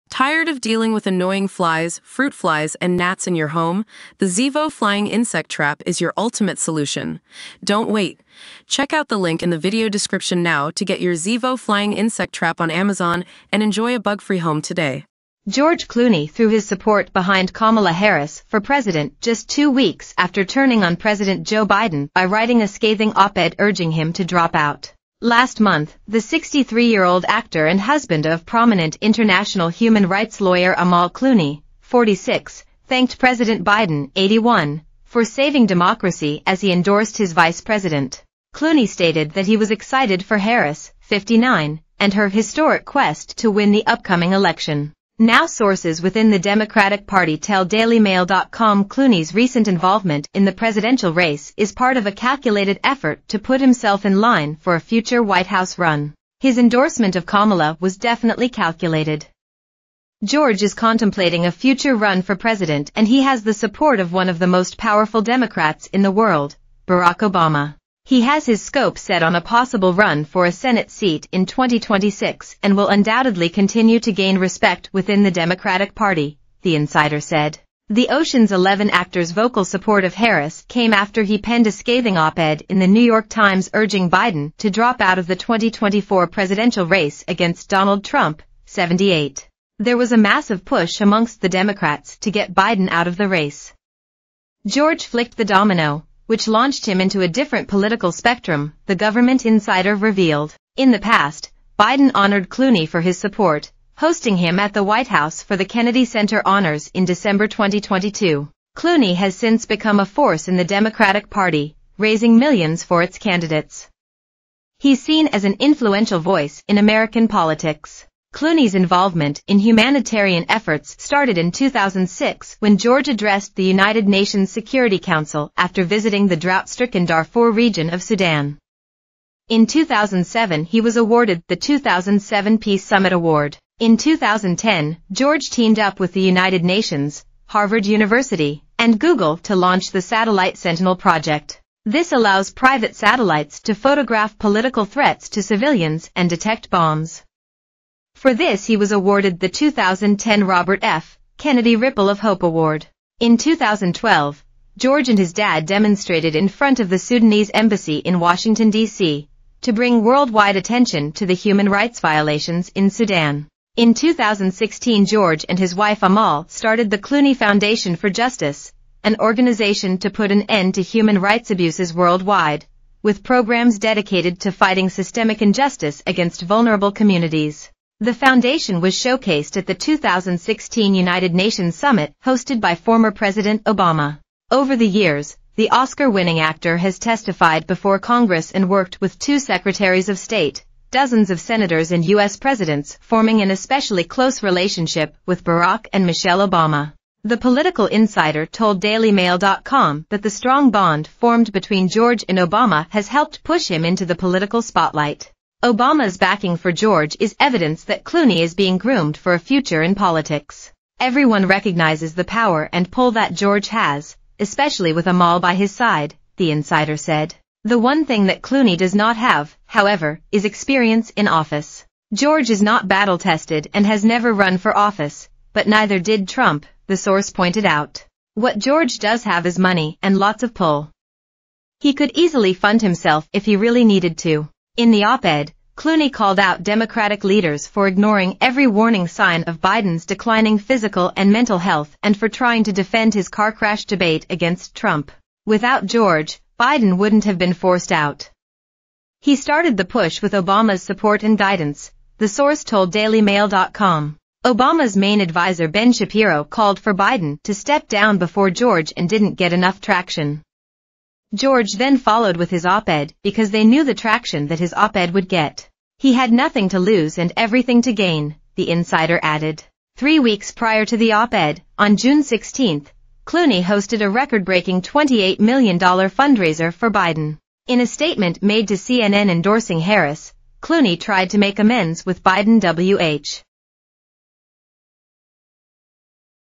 EXCLUSIVE Inside George Clooney's own bid to become PRESIDENT: How the actor used his "calculated"
EXCLUSIVE Inside George Clooney's own bid to become PRESIDENT: How the actor used his "calculated" anti-Biden op-ed and support for Kamala to try to launch his own political career